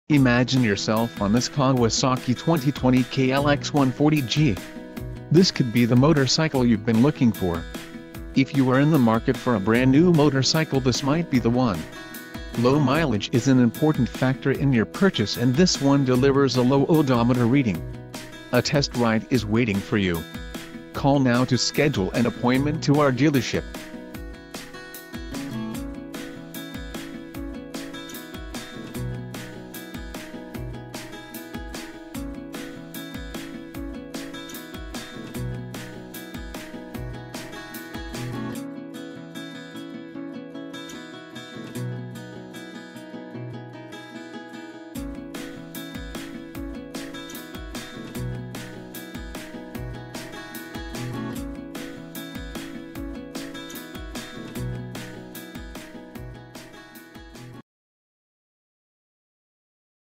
2020 Kawasaki KLX®140G
OFF-ROAD LIKE NEVER BEFORE
With an electric start, and confident handling, KLX and #174;140, KLX and #174;140L and KLX and #174;140G motorcycles are the ideal entry into off-road riding. The easy-to-ride KLX140 series offer a 144cc engine, plush suspension and a push button electric start, making for great trailblazers.

Features

CONFIDENT HANDLING
LIGHTWEIGHT RIGID FRAME: The lightweight box-section perimeter frame is made from high-tensile steel for exceptional rigidity and strength. The narrow profile contributes to an easier rider grip and smoother movement on the bike. Working together, the highly capable suspension and lightweight chassis deliver handling that exudes confidence.

RELIABLE BRAKING
FRONT AND REAR DISC BRAKES: Front and rear disc brakes provide strong progressive stopping power.